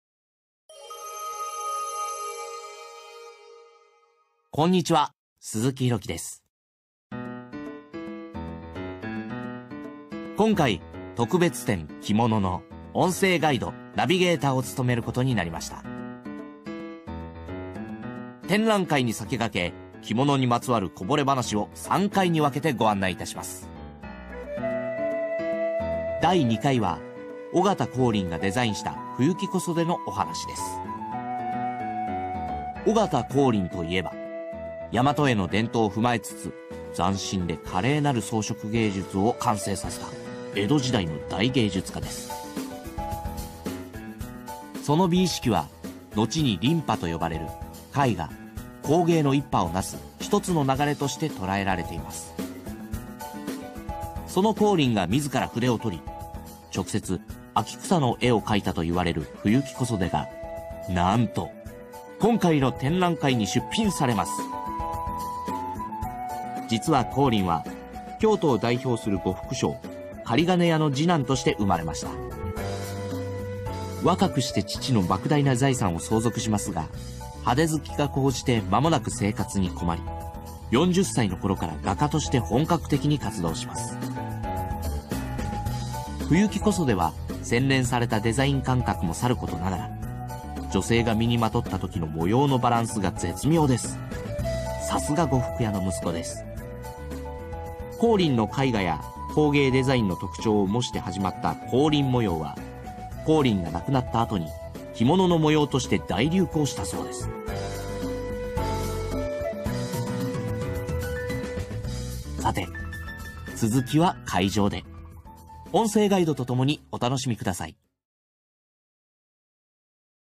第2回「呉服屋の息子―尾形光琳デザインの小袖」
きものにまつわるこぼれ話を 鈴木拡樹 さんに朗読していただきました！